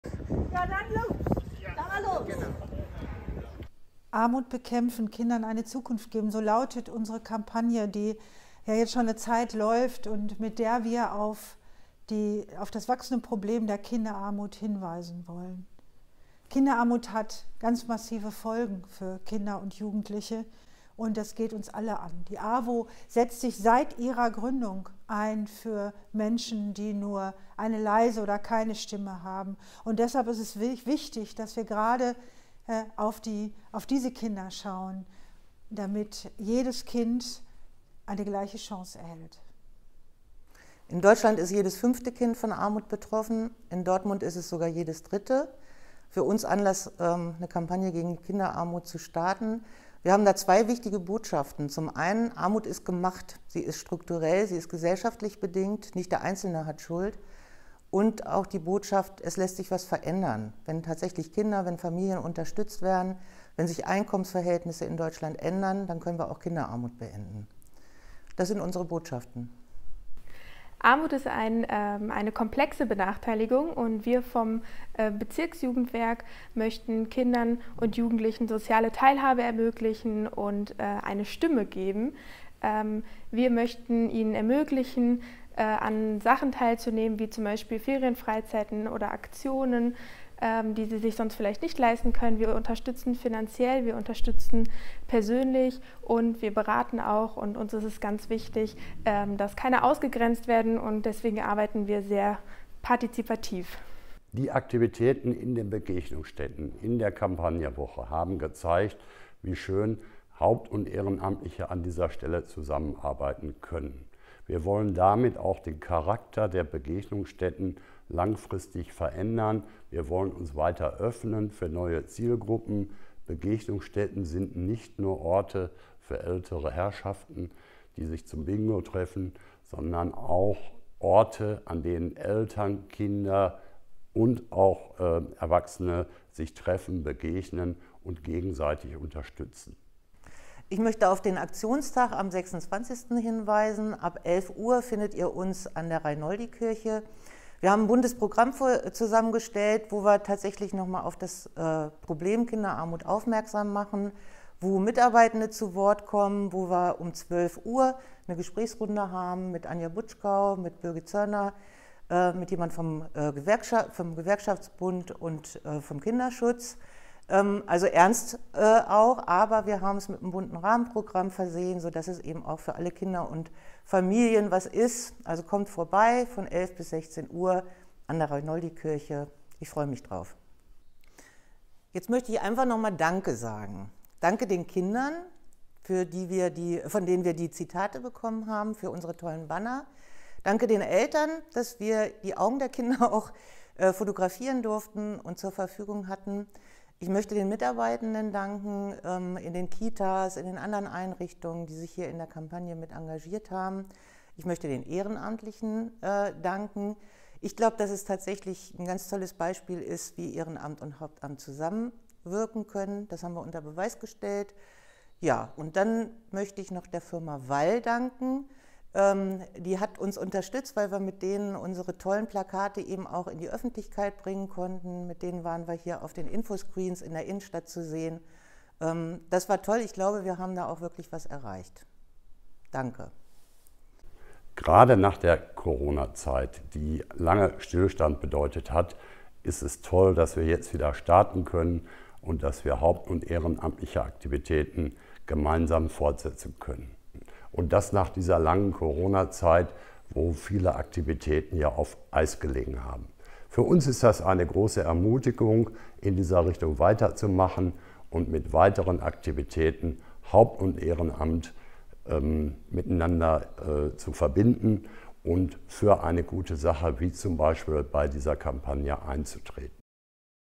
Demonstration gegen Kinderarmut in Dortmund - „Armut bekämpfen – Kindern eine Zukunft geben!“
Wir von der AWO haben heute mit einer Demonstration in der Dortmunder City auf unsere Kampagne „Armut bekämpfen – Kindern eine Zukunft geben!“ auf das wachsende Problem der Kinderarmut in Dortmund und Deutschland aufmerksam gemacht. Am Samstag ab 11 Uhr machen wir dazu auch einen Aktionstag mit Familienfest an der Reinoldikirche. Bitte weitersagen - ihr seid herzlich willkommen.